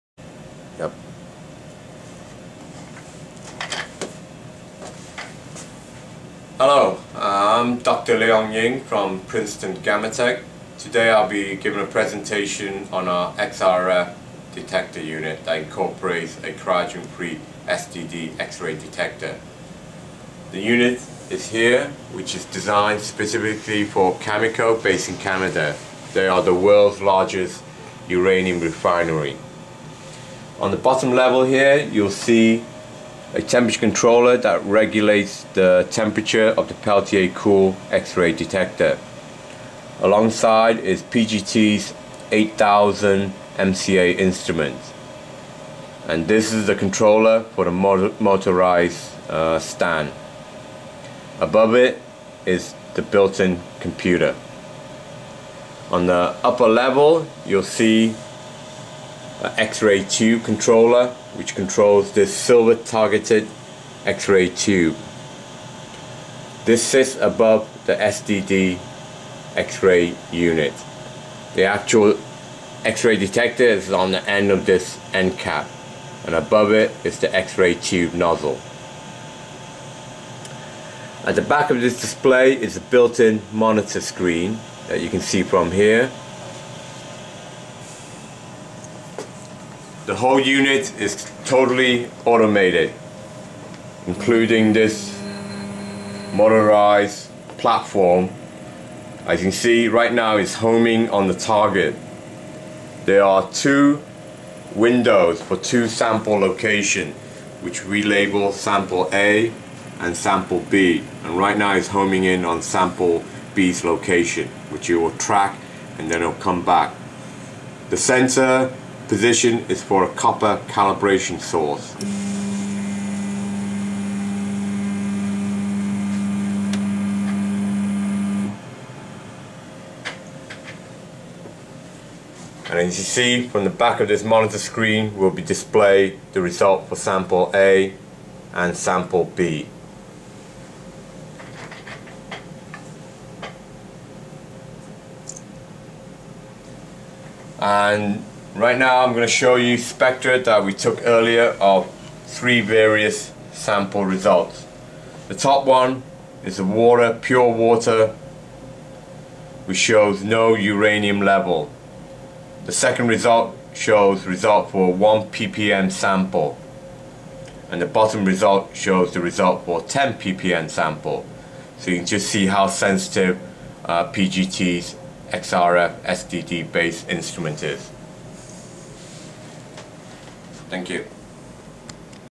PGT X-Ray Fluorescence Silicon-Drift Detector
Automated x-ray fluorescence (XRF) analyzer incorporating a cryogen-free silicon-drift detector (SDD) to be used as a process control system at Cameco's uranium refinery.